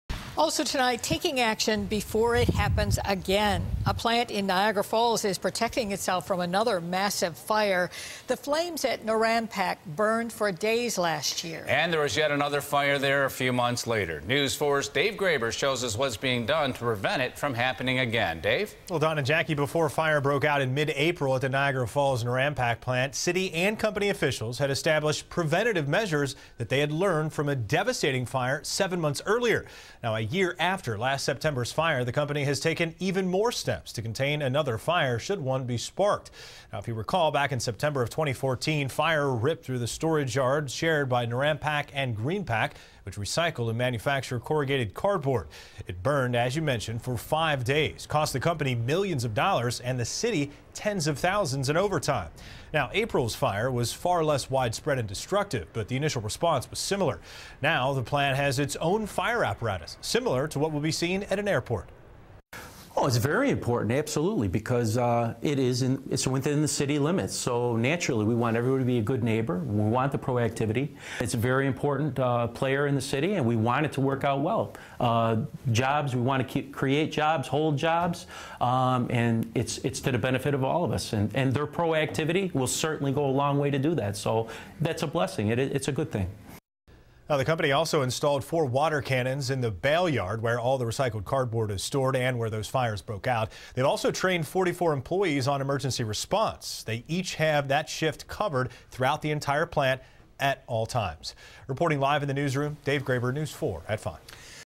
Niagara Falls company working to prevent costly fires
Norampac, Greenpac bought fire apparatus, installed water canons